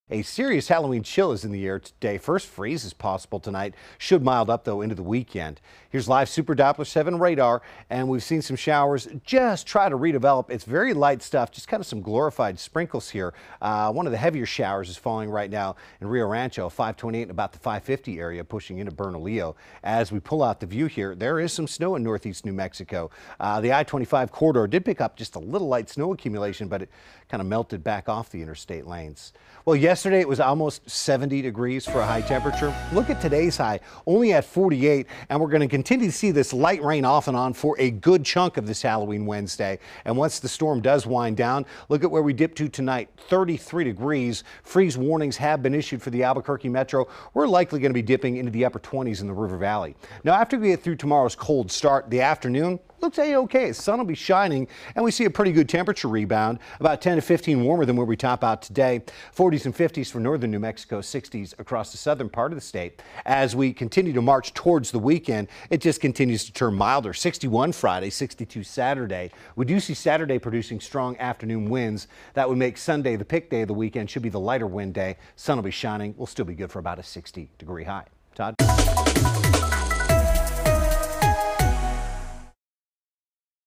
A spooky cold and wet Halloween
Winter storm brings serious Halloween chill to the state, along with rain and snow.  Tonight will be the new coldest night of the fall for many New Mexico cities.